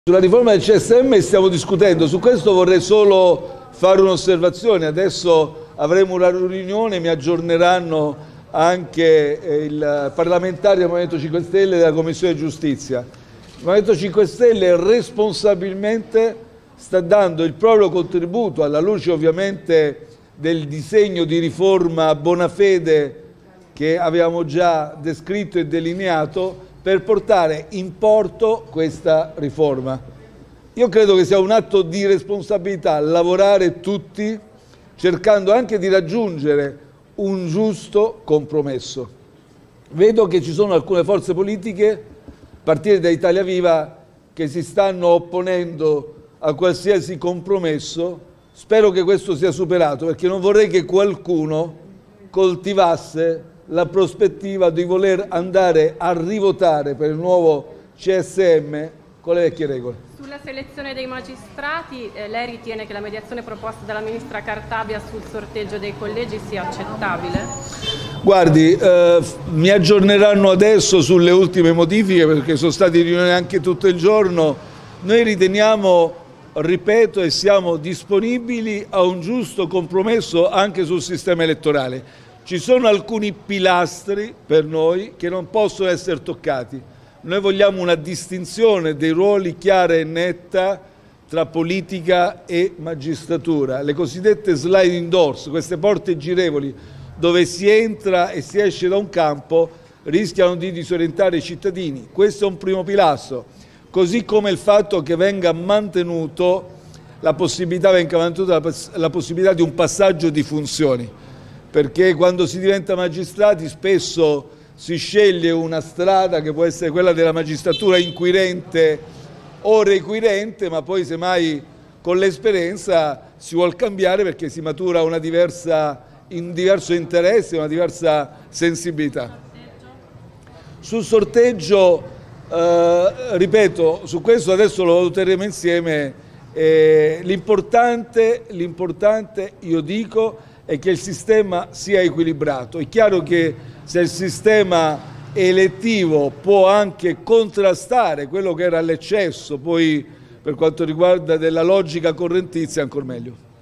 Csm, Conte: "Lavorare a compromesso atto responsabilità"
"Importante avere sistema equilibrato" commenta il leader M5S

POLITICA (Roma). "Il m5s responsabilmente sta dando il proprio contributo per portare in porto questa riforma del Csm" commenta Giuseppe Conte. "E' un atto di responsabilita' lavorare tutti per raggiungere un compromesso, vedo che Iv si oppone, non vorrei che qualcuno volesse andare a votare al rinnovo del Csm con le vecchie norme" continua Conte. "Siamo disponibili a un giusto compromesso anche sulle regole elettorali. Vogliamo una distinzione chiara e netta fra politica e magistratura, questo e' un primo pilastro, come quello sul passaggio delle funzioni. L'importante e' che il sistema sia equilibrato" conclude. (Vanessa Ciccarelli/alanews)

stiamo discutendo su questo vorrei solo fare un osservazione adesso avremo la riunione mi aggiorneranno anche il parlamentare Movimento 5 Stelle commissione giustizia 5 Stelle responsabilmente sta dando il proprio contributo alla luce ovviamente del disegno di riforma Bonafede e abbiamo già descritto è delineato per portare in porto questa riforma Io credo che sia un atto di responsabilità lavorare tutti cercando anche di aggiungere un giusto compromesso vedo che ci sono alcune forze politiche partire da Italia viva che si stanno ponendo a qualsiasi compromesso Spero che questo sia superato perché non vorrei che qualcuno coltivasse la prospettiva di voler andare a rivotare per nuovo CSM Collecchio sulla selezione dei magistrati e lei ritiene che la mediazione proposta della ministra cartabia sul sorteggio dei collegi si accende notabile guardi mi aggiorneranno adesso sulle ultime Dimmi perché sono stati lì non è neanche tutto il giorno noi riteniamo ripeto e siamo disponibili a un giusto compromesso anche sul sistema elettorale Ci sono alcuni pila per noi che non posso essere toccati noi vogliamo una distinzione dei ruoli Chiara è netta tra politica e magistratura le cosiddette slide in Doors queste porte girevoli dove si entra e si esce da un campo rischiano di disorientare i cittadini Questo è un primo Pilastro così come il fatto che venga mantenuto la possibilità la possibilità di un passaggio di funzioni Perché quando si diventa magistrati spesso si sceglie una strada che può essere quella della Magistratura inquirente segui dente ma poi semmai con l'esperienza si vuol cambiare perché si matura una diversa in diverso interesse ma diversa sensibilità su sorteggio Ripeto su questo adesso lo tiriamo insieme L'importante L'importante io dico è che il sistema sia equilibrato È chiaro che se il sistema elettivo può anche contrastare quello che era all'eccesso Poi per quanto riguarda della logica correntezza ancor meglio